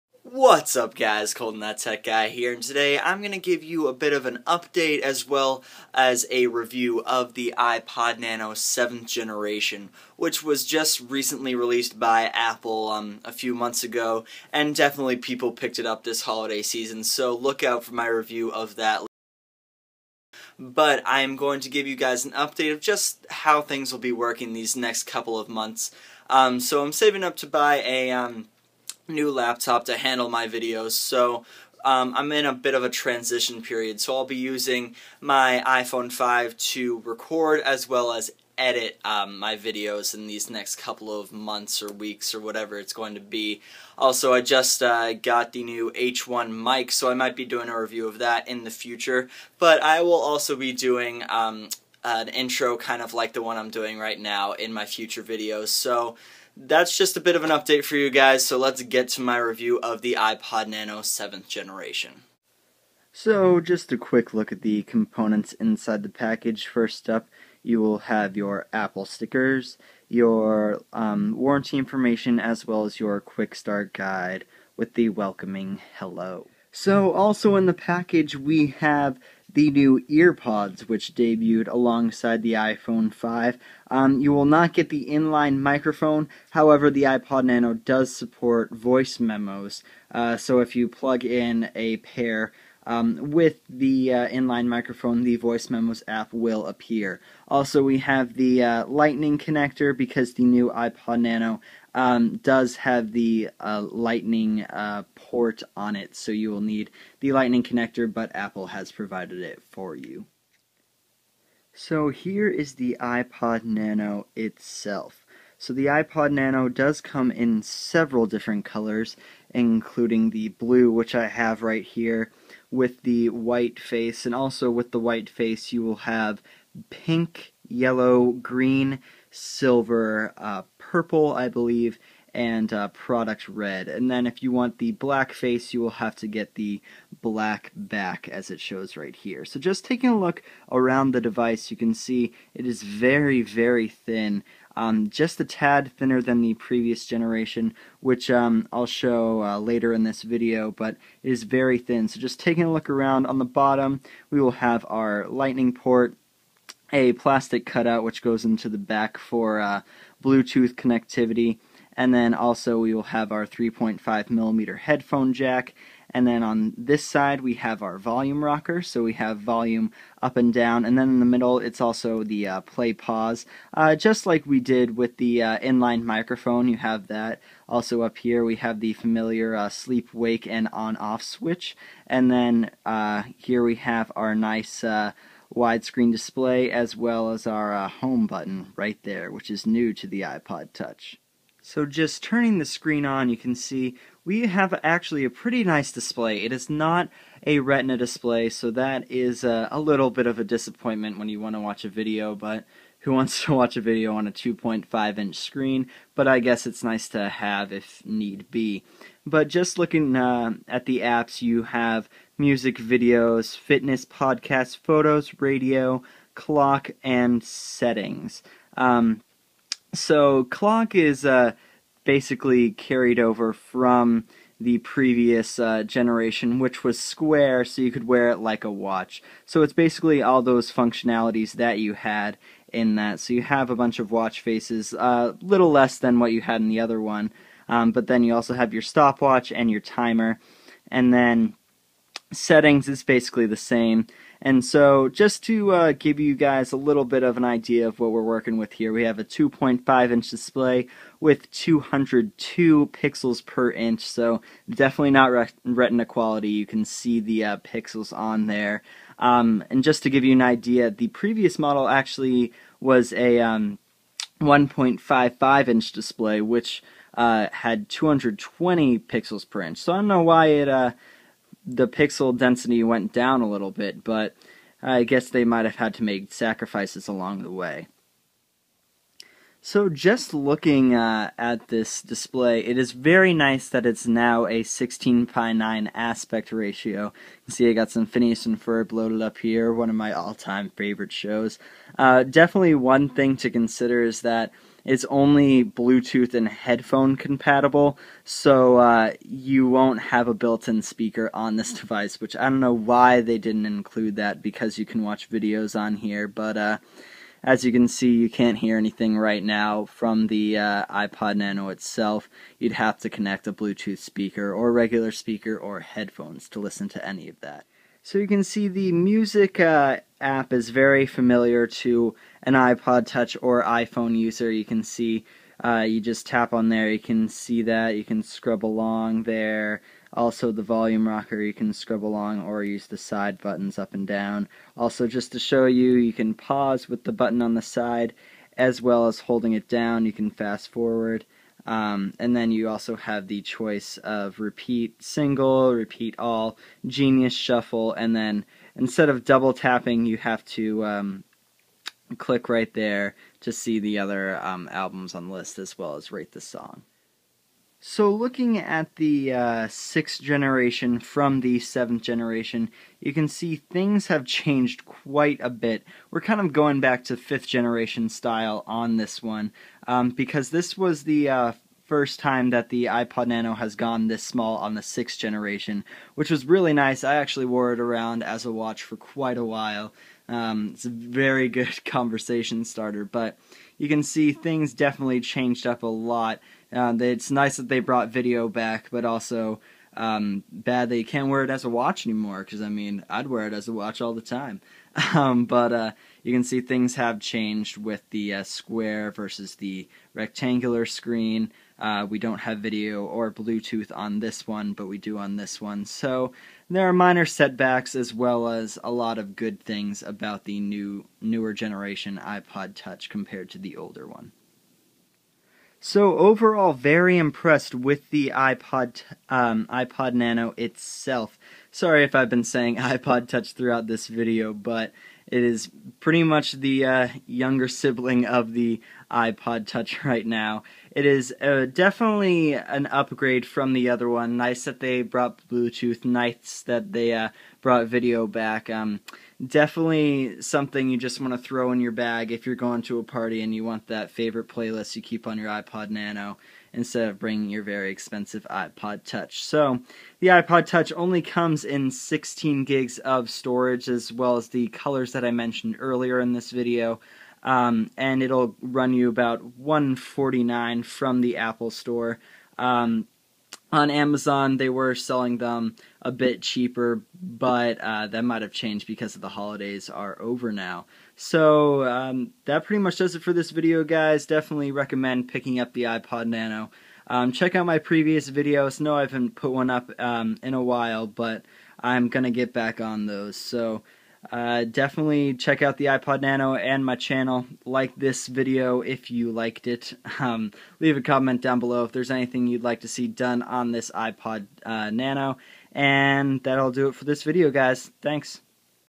iPod Nano 7th Generation (iPod Nano 7G): Overview & Review
Here is a quick overview and review of the iPod Nano 7th Generation!

But remember you have to be a follower to be considered a participant!